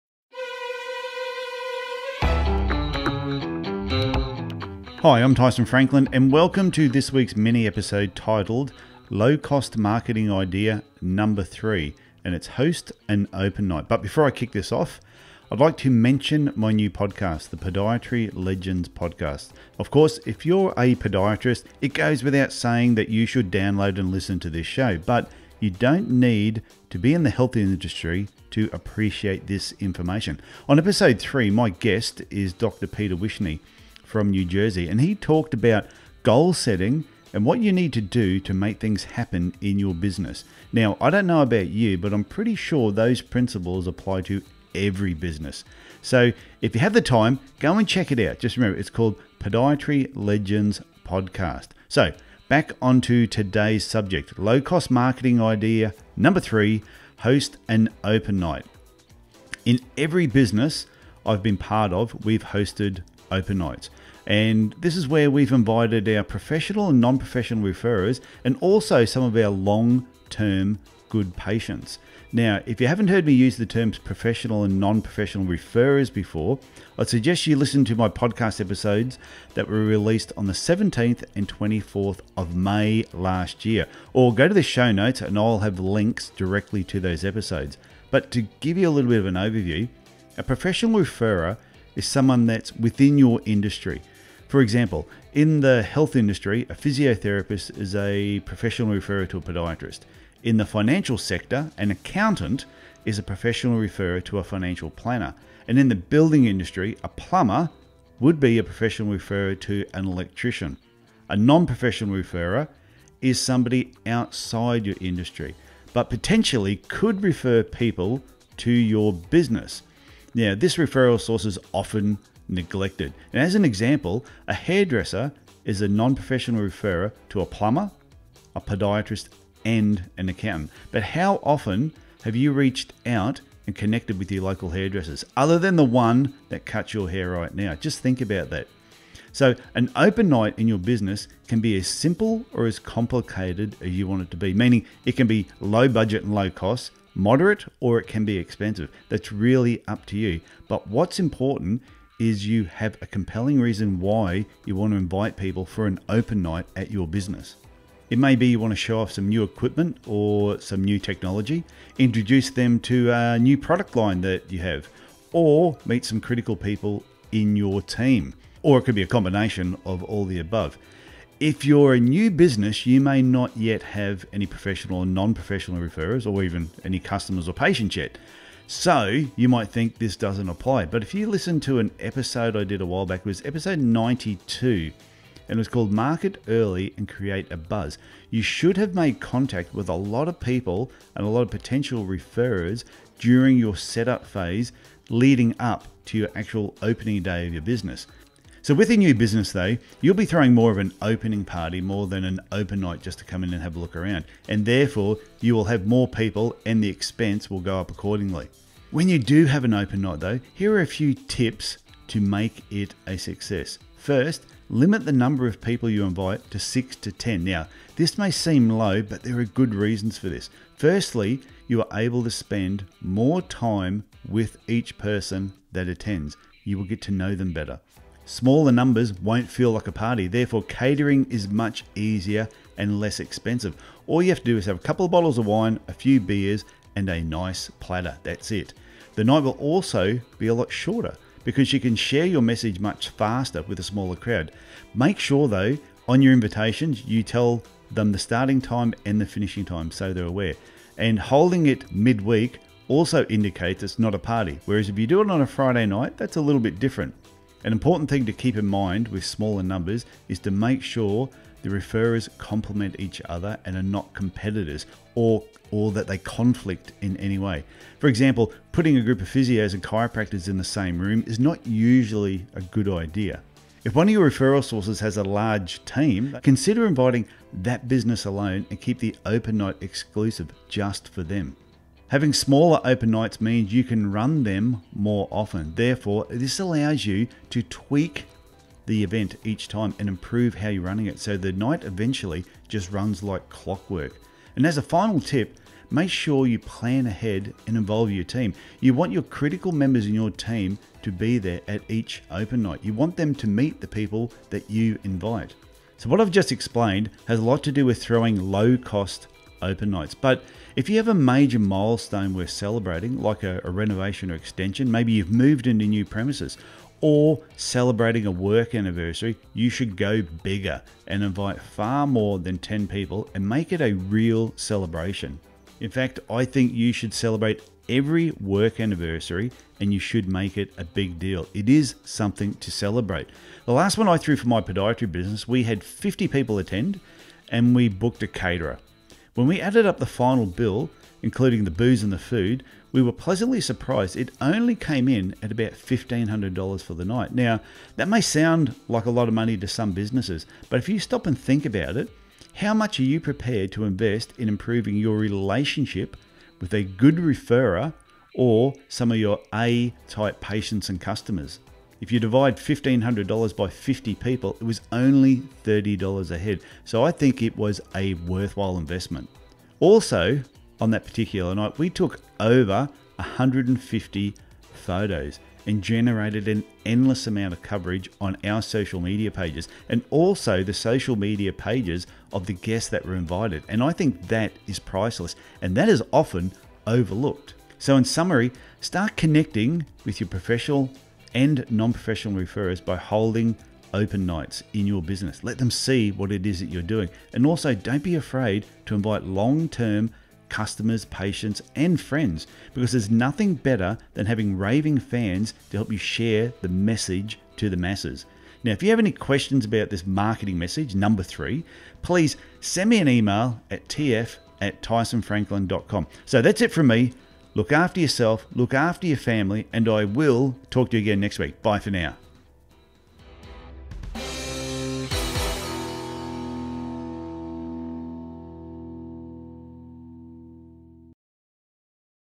145: Low-Cost Marketing Idea #3: Host Open Nights
In every business, I’ve been part of we've hosted an open night, where we’ve invited our professional and non-professional referrers, and long-term A-type patients. It a great way to build trust and raving fans.

 Now if you haven’t heard me use the terms professional and non-professional referrers before I’d suggest you listen to my podcasts episodes released on the 17th and 24 May 2018.

  Marketing Pillar One: Professional Referrers

  Marketing Pillar Two: Non-Professional Referrers

 But to give you a quick overview:

 A Professional Referrer: Is someone within your industry. For example, in the health industry a Physiotherapist is a professional referrer to a Podiatrist, in the financial sector an Accountant is a professional referrer to a Financial Planner, and in the building industry, a Plumber is a professional referrer to an Electrician.

 A Non-Professional Referrer: Is someone outside your industry but potentially could refer people to your business. This referral source is often neglected. As an example, a Hairdresser is a non-professional referrer to a Plumber, Podiatrist or Accountant, but how often have you reached out to connect with your local hairdressers, other than the one you visit yourself.

 Simple Or Complex

 An open night in your business can be as simple or as complicated as you want, meaning it can be low-cost, moderate or expensive, that's really up to you, but what’s important is you have a compelling reason why you want to invite people for an open night.

 It may want to:

  Show off some new equipment or technology Introduce them to a new product line Have them meet critical people in your team Or it could be a combo of all of the above  Are You A New Business?

 Now if you’re a new business you may not yet have any professional or non-professional referrers yet, well so you would think, but if you listened to  Episode 92 – Market Early &amp; Create A Buzz you should have made contact with a lot of potential referrers during your setup phase leading up to your opening day.

 With a new business though you would be throwing more of an opening party than an open night and therefore there will be more people, and the expense will go up accordingly.

 Open Night Tips

 When you do have an open night, here are a few tips to make it a success:

  Limit the number of people you invite (6 - 10 people). This may seem low, but there are good reasons for this.  You are able to spend more time with each person that attends. Smaller numbers won’t feel like a party; therefore catering is much easier and less expensive. All you’ll need is a couple of bottles of wine, a few beers and a nice platter. The night will also be much shorter because you can share your message much faster.   Make sure you put the time on your invitation (6-7pm). Holding it mid-week also indicates it is not a party. Friday nights are a little different. An important thing to keep in mind with smaller numbers is to make sure the referrers compliment each other and are not competitors, or conflict in any way.  Putting Physiotherapists and Chiropractors in the same room is not usually a good idea.   If one of your referral sources has a large team, consider inviting that business and keep the open night exclusive for them. Make sure you plan ahead and involve your team. You want the critical members of your team at each open night.  Improve As You Go

 Having smaller open night means you can run them more often and therefore allows you time to tweak and improve how the night runs until it runs like clockwork.

 Celebrate Major Milestones

 So, what I’ve just explained has to do with throwing a low-cost open night, but if you have a major milestone worth celebrating, like a renovation or extension, moved into new premises or celebrating a work anniversary, you should go bigger and invite far more than ten people and make it a real celebration.

 In fact, I think you should celebrate every work anniversary and you should make a big deal of it.

 $1500 Was a Great Investment

 The last one I threw for my podiatry business, we had 50 people attend, and we booked a caterer. When we added up the final bill, including booze and food, we were pleasantly surprised it only came to $1500.

 $1500 may sound like a lot to some businesses, but if you stop and think about it, how much are you prepared to invest in improving your relationship with a good referrer or A-Type patient/customer?

 $1500 divided by 50 people is only $30 per head, so I think that is a worthwhile investment.

 Social Media Exposure

 We also took over 150 photos on the night and generated an endless amount of coverage on our social media pages and also the social media pages of our guests, which was priceless.

 So in summary, start connecting with your professional and non-professional referrers by holding opening nights in your business, and also don't be afraid to invite...